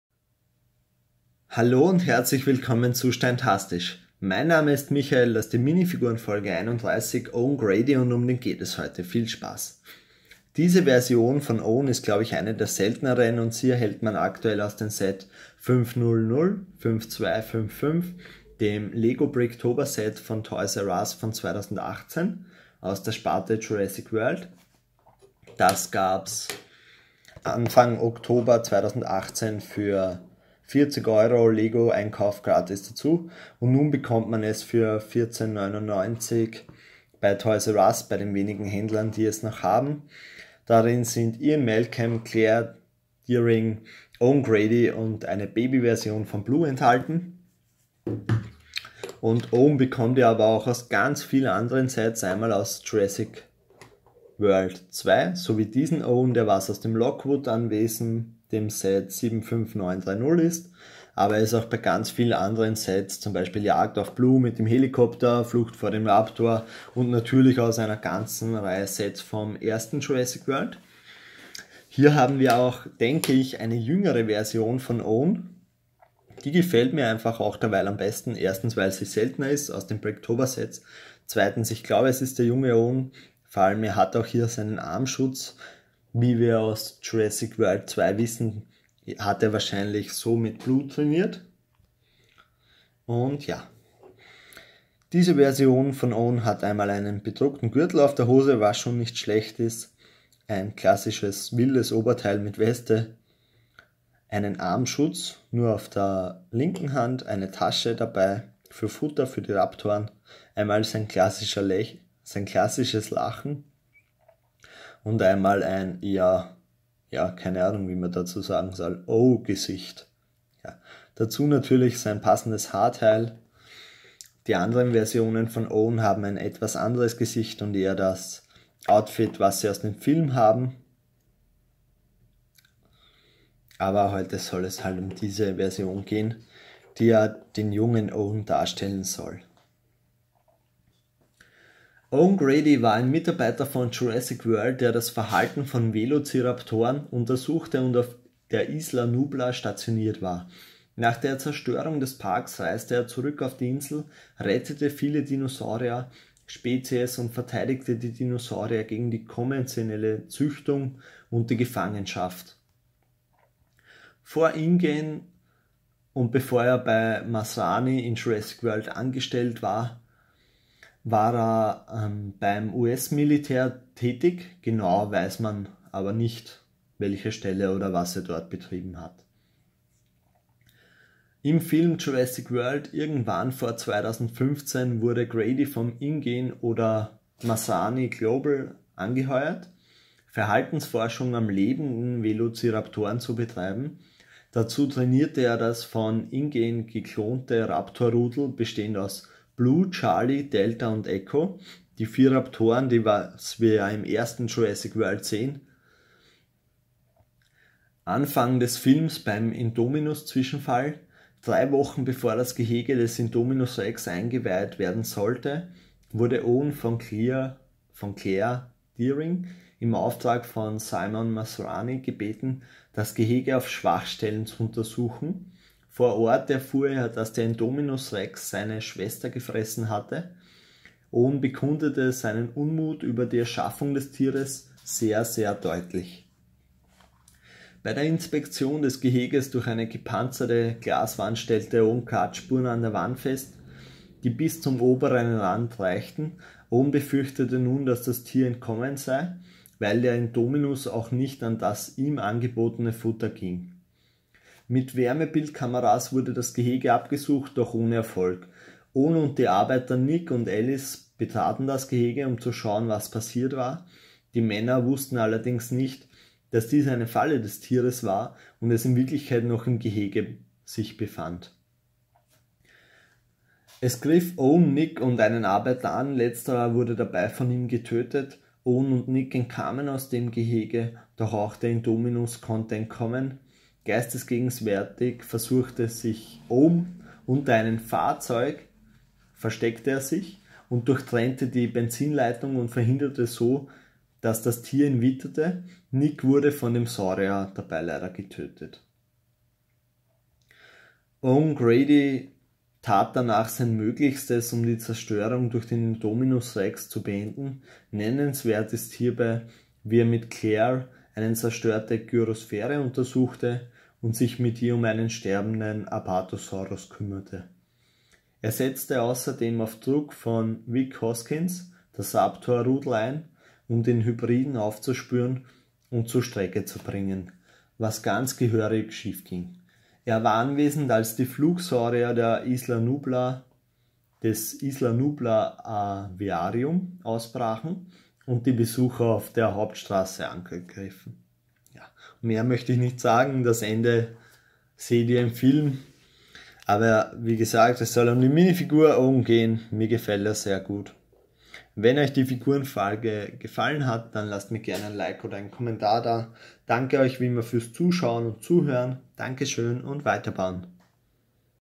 LEGO Minifigurenfolge 31 /Owen Grady
Wo und wie bekommt man die Figur: Aus dem Set 5005255 das es aktuell nur bei ToysRus zu haben gibt.
Preis: 14,99€

LEGO ist eine eingetragene Marke der Lego Gruppe.